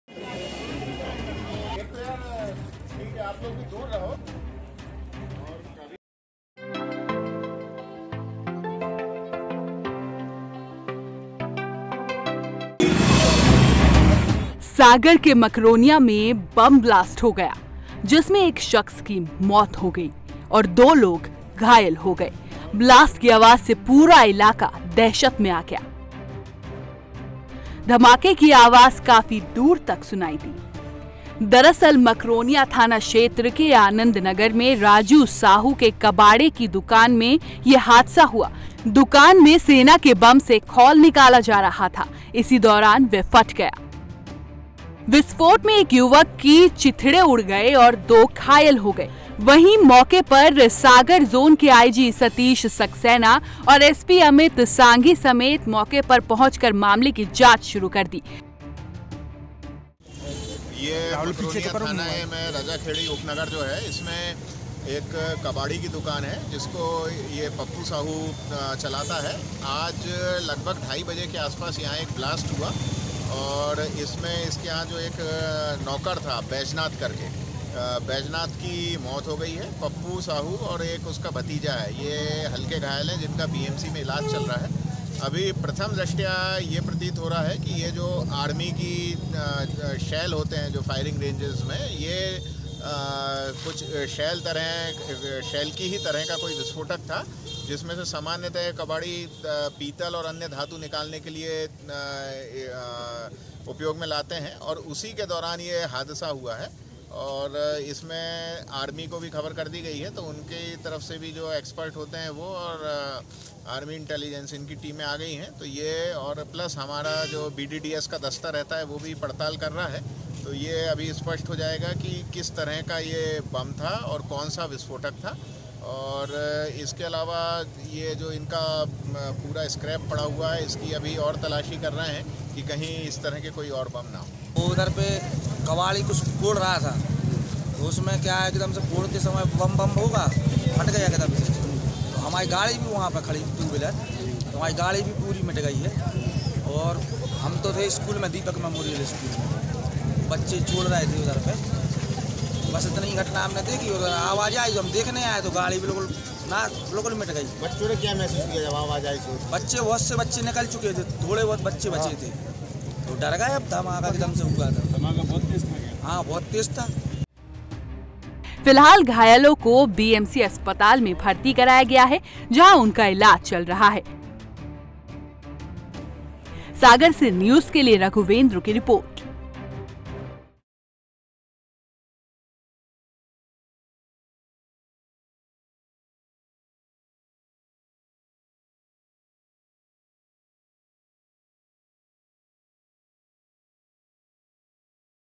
Sagar : कबाड़ की दुकान में सेना के बम का कवर उतारते वक्त ब्लास्ट, 1 की मौत
सागर के मकरोनिया में बम ब्लास्ट हो गया...जिसमें एक शख्स की मौत हो गई...और दो लोग घायल हो गए...ब्लास्ट की आवाज से पूरा इलाका दहशत में आ गया...धामाके की आवाज काफी दूर तक सुनाई दी...दरअसल मकरोनिया थाना क्षेत्र के आनंदनगर में राजू साहू के कबाड़े की दुकान में यह हादसा हुआ...दुकान में सेना के   बम से खोल निकाला जा रहा था...इसी दौरान वह फट गया...विस्फोट में एक युवक के चिथड़े उड़ गए और दो घायल हो गए...वहीं, मौके सागर जोन के आईजी सतीश सक्सेना और एसपी अमित सांघि समेत मौके पर पहुंचकर मामले की जांच शुरू कर दी...।   About Channel: